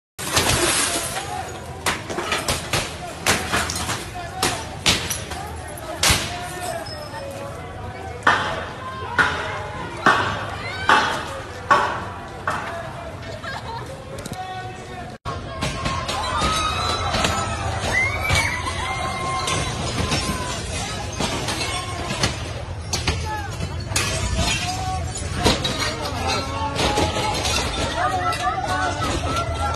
“¡A Giovanni lo mató el Estado!”, protestan en CDMX; grupos causan destrozos en Reforma
Un grupo de individuos encapuchados comenzaron a golpear los cristales de tiendas de autoservicio, bancos y puestos callejeros.